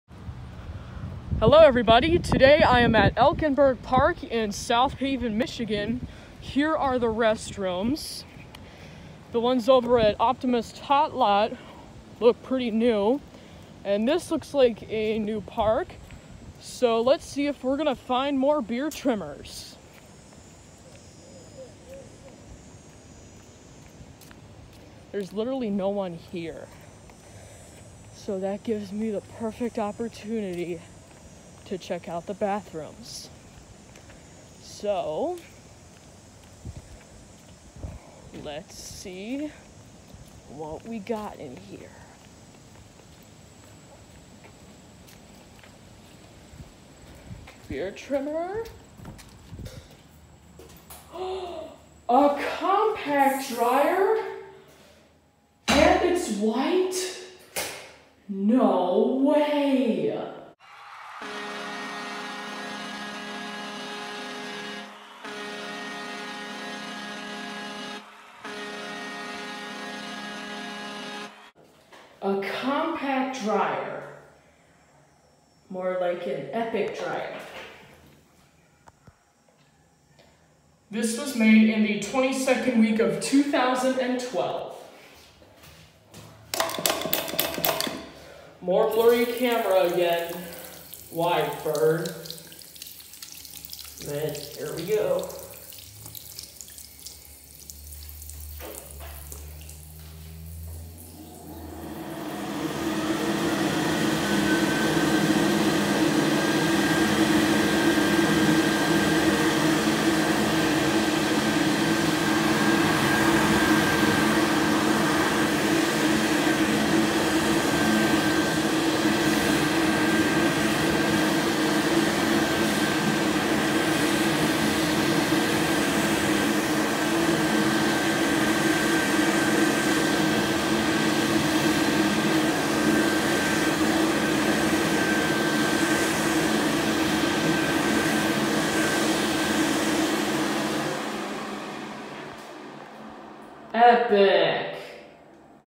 Bobrick CompacDryer @ Elkenburg Park - South Haven, MI
How do I find Bobricks so common in Michigan? Next I need to find an Elan.